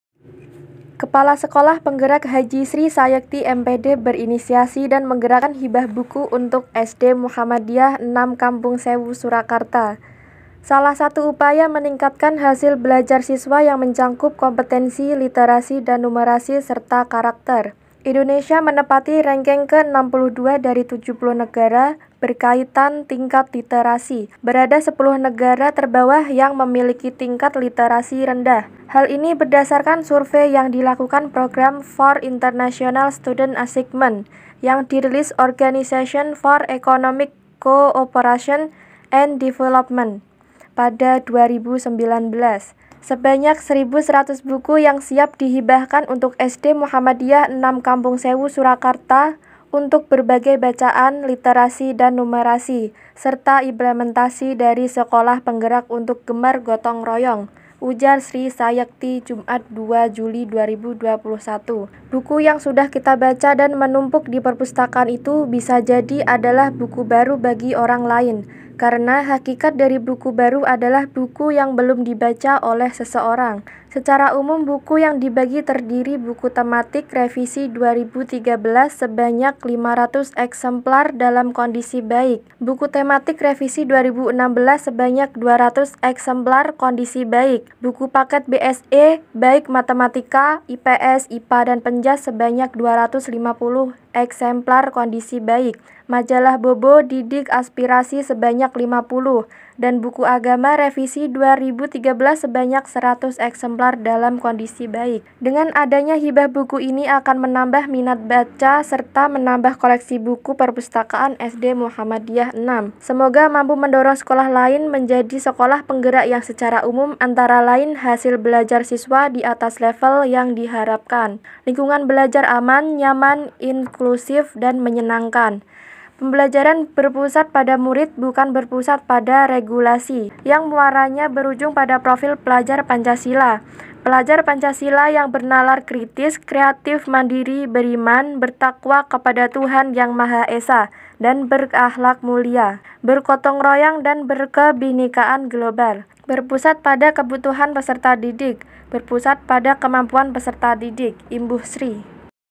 Gerakan SD Muh 1 Ketelan Hibahkan 1100 Buku
Kepala Sekolah Penggerak Hj Sri Sayekti MPd berinisiasi dan menggerakkan hibah buku untuk SD Muhammadiyah 6 Kampung Sewu Surakarta, salah satu upaya meningkatkan hasil belajar siswa yang mencakup kompetensi literasi dan numerasi serta karakter.